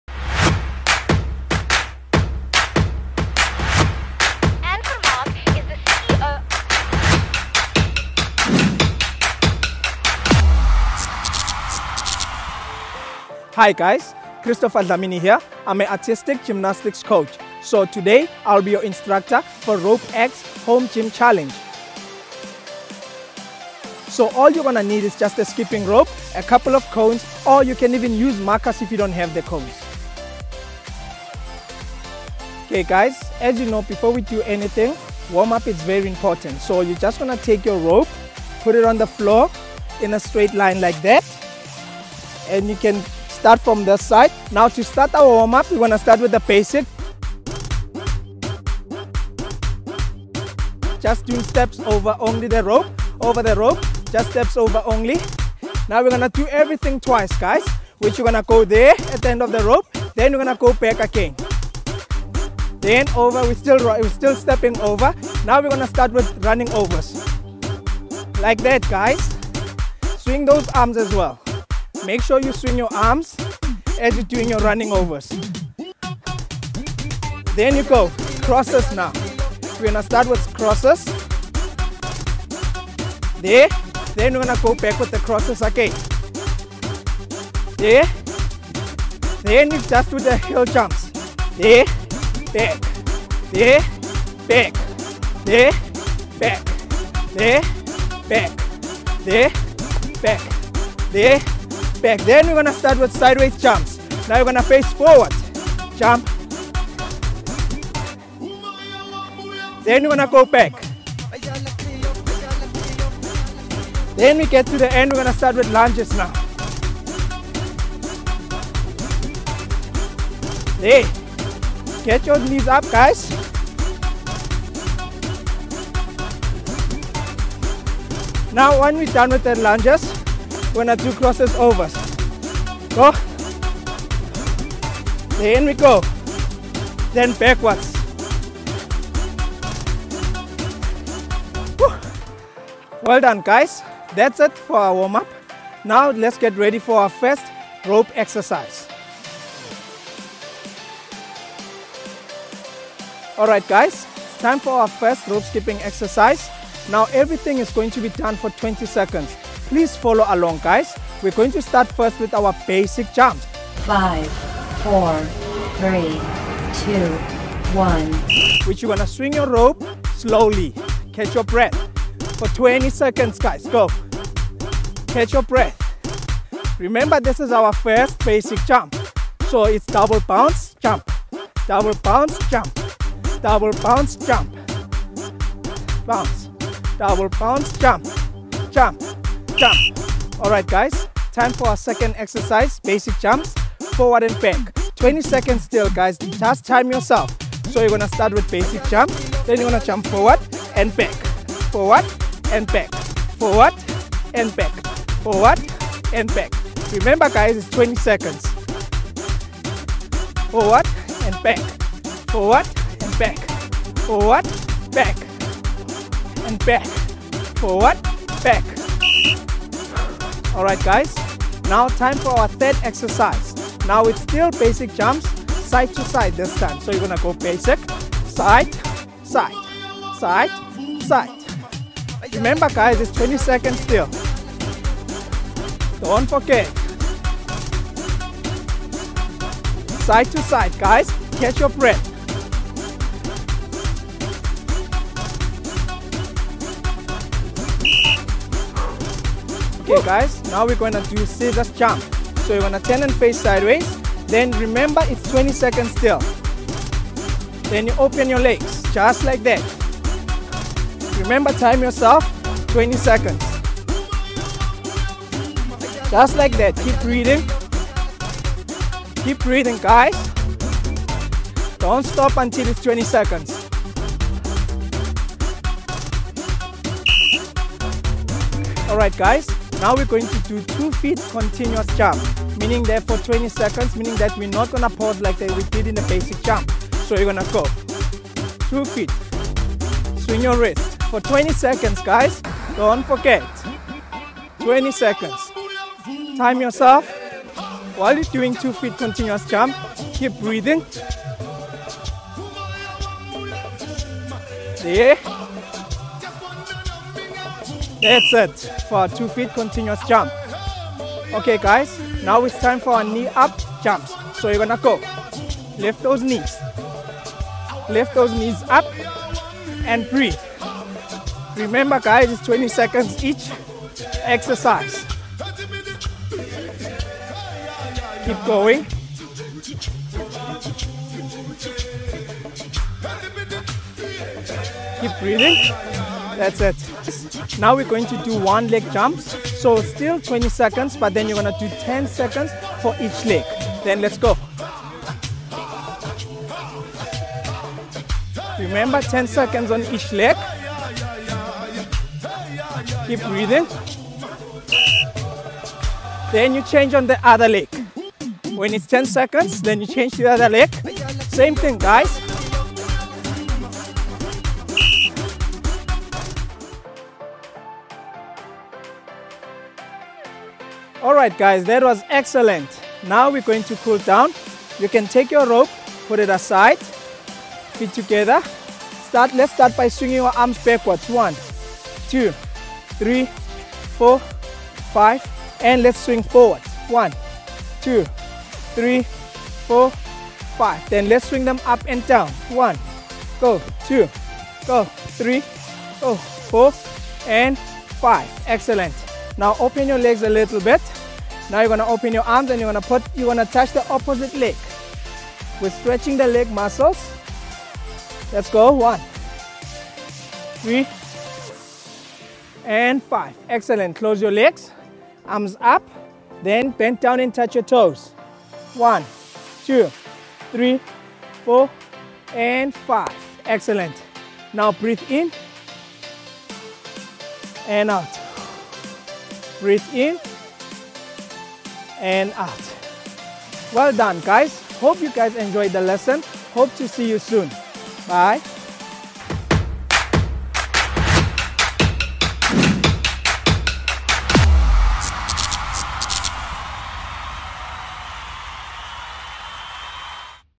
ROPE-X Home Gym workout with Christopher Dlamini
Let's keep warm by getting fit this winter! Join Christopher Dlamini as he guides us through the ROPE-X workout.  All you need is a skipping rope and some space to get your body moving. Did you know that just 10minutes of skipping is worth 40minutes of jogging?!